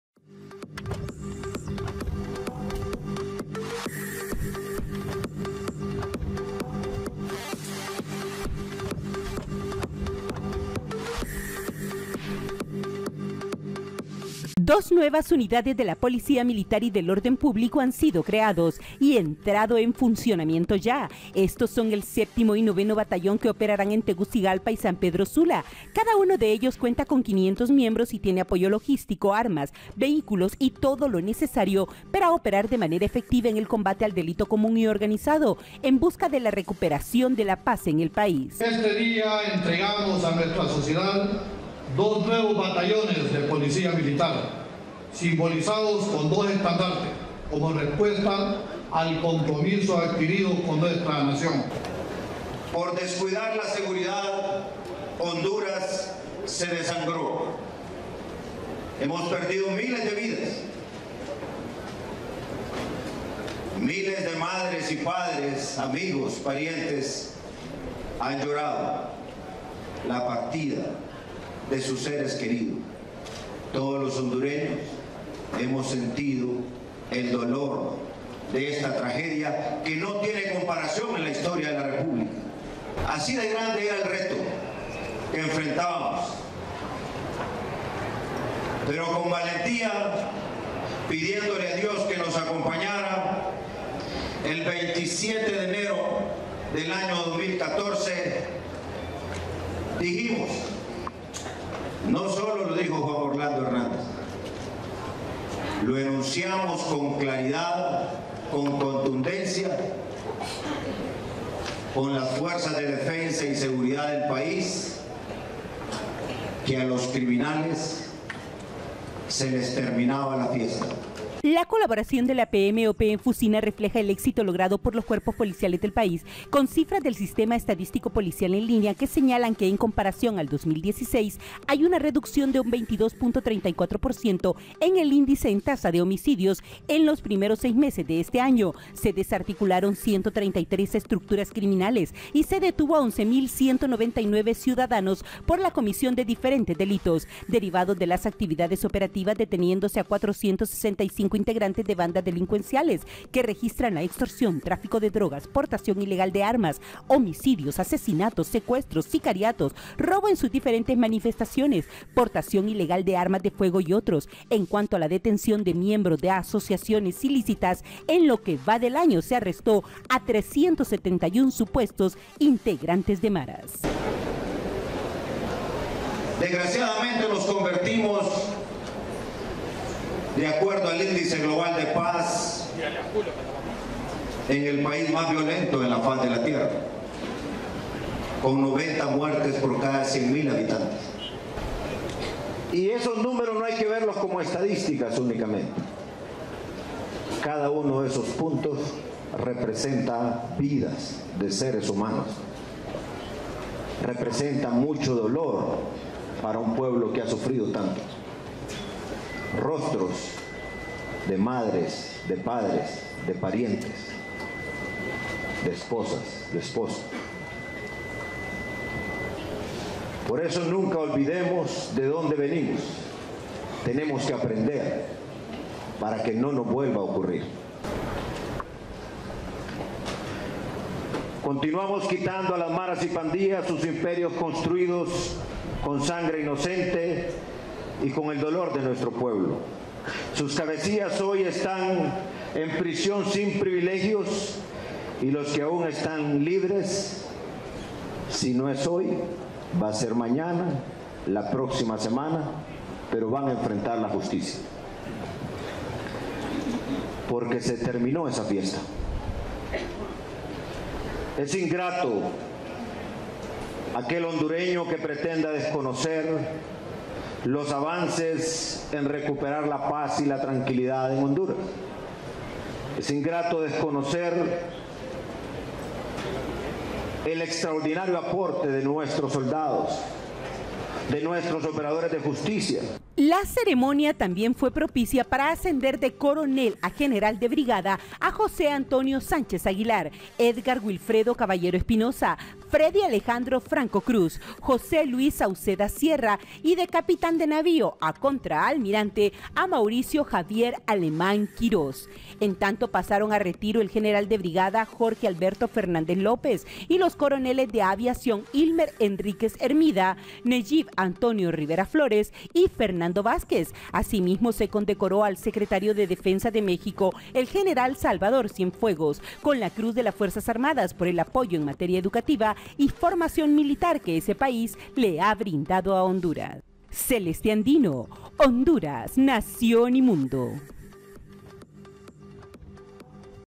Nuevos Batallones PMOP
2 nuevos batallones de la PMOP, ascensos, condecoraciones y retiros de las FFAA preside el Presidente Juan Orlando Hernández